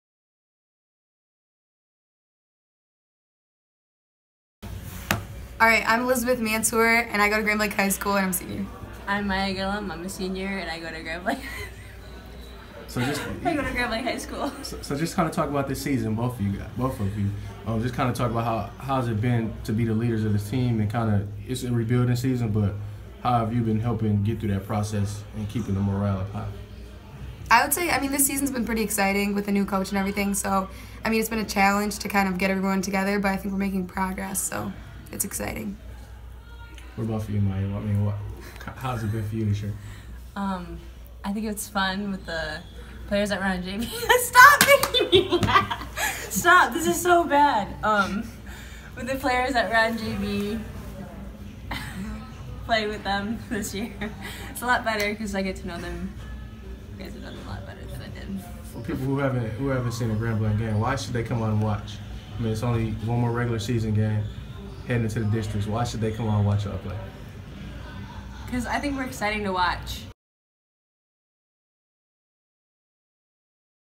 Grand Blanc senior basketball players discuss 2016-17 season
Grand Blanc seniors Elizabeth Mansour and Maija Gillum describes how it feels to lead the girls basketball team in the 2016-17 season.